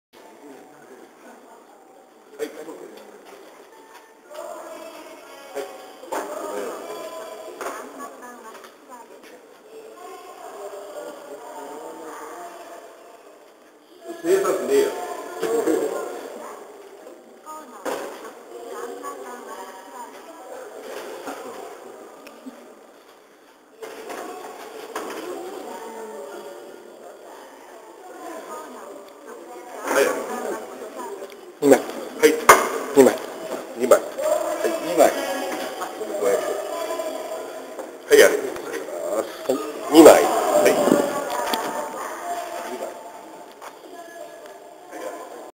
2010.7.17成田湯川駅の記念乗車券発売
2010年7月17日京成電鉄成田空港線成田スカイアクセスが開業しました。唯一の新規開業駅である成田湯川駅で記念乗車券が発売されました。本動画はその発売の様子です。記念乗車券は3種類発売されましたが成田湯川駅分のみ唯一の硬券でした。他2種は京成と北総鉄道方面相互のもの（軟券）と、京成上野駅から成田空港までの乗車券・特急券がセットになった軟券でした。